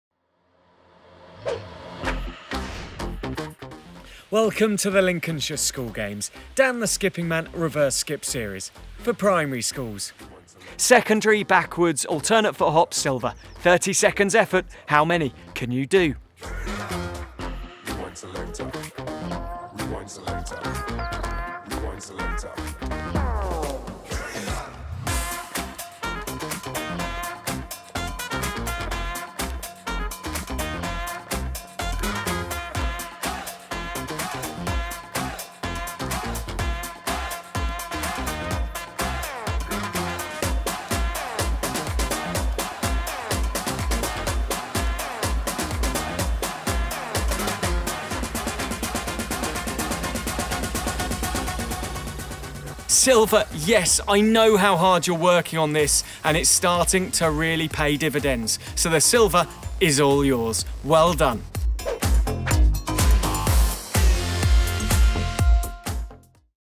DTSM Reverse Skip SEC – Alternate Foot Hop Silver
Dan the Skipping Man® challenges you to reach the Silver level 30 seconds of backwards Alternate Foot Hop